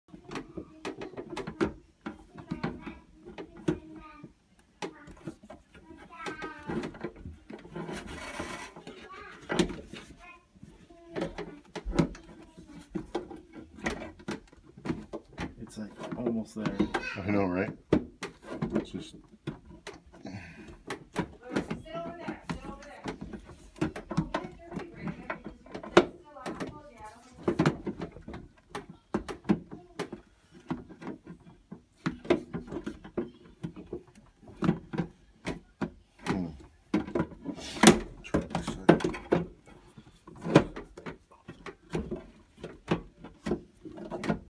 Real life tavern puzzle with laptop and in-room hotel safe
TIFU by getting my laptop stuck in a hotel safe.